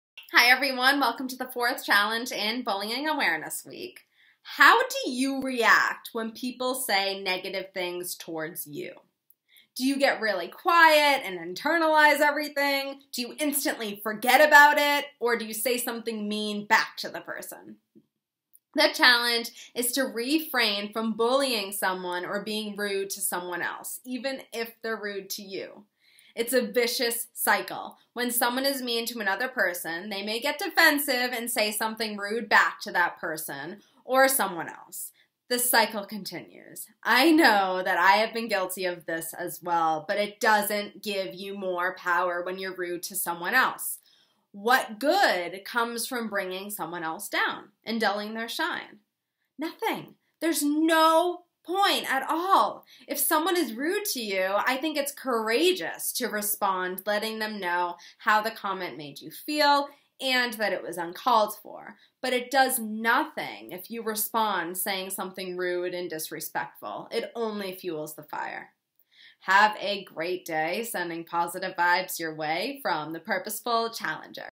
Break The Bullying Cycle
How do you react when people say negative things towards you? Do you get quiet and internalize everything? Do you instantly forget about it? Do you say something mean back to the person?

This video explains one way to break the bullying cycle.

Sending positive vibes your way from The Purposeful Challenger